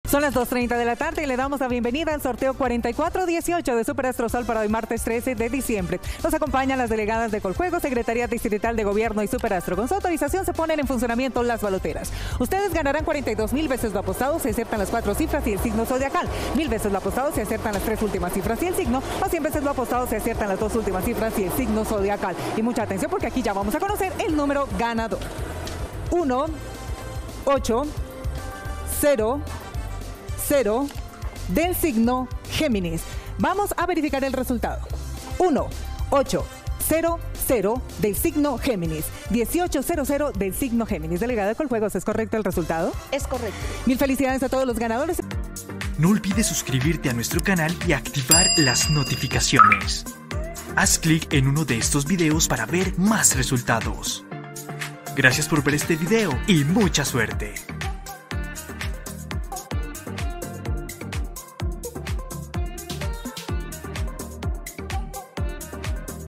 Resultados ASTRO SOL del MARTES 13 de DICIEMBRE de 2022 🌟💰🤩 (Número ganador del Super Astro)
¡Bienvenido! Aquí tenemos el resultado del último sorteo del SUPER ASTRO SOL de hoy MARTES 13 de DICIEMBRE de 2022, también puedes ver al final los resultados de las loterías, chances y sorteos más importantes de Colombia, en este video podrás ver el número ganador.